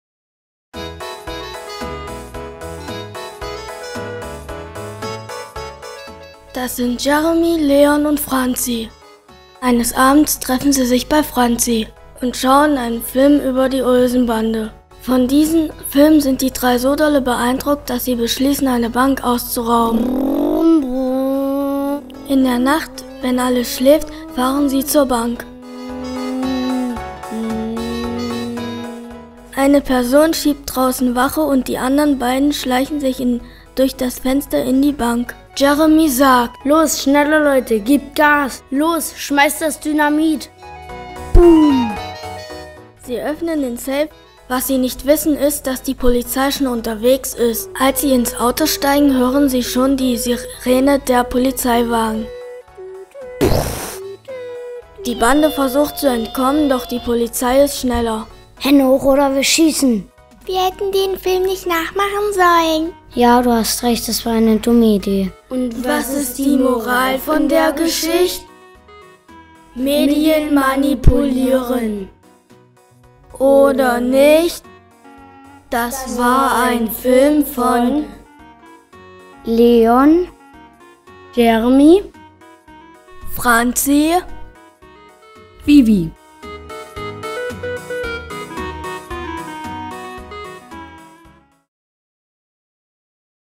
Wie spielen Olsen Bande
Ein kritischer Film von Leon, Jeremy, Franzi und Vivi über Medien und deren Wirkung auf die Rezipientenam Beispiel der Olsen Bande. Produziert von Kindern aus Dessau und Umgebung während des Quetzer Kunstsommers 2010.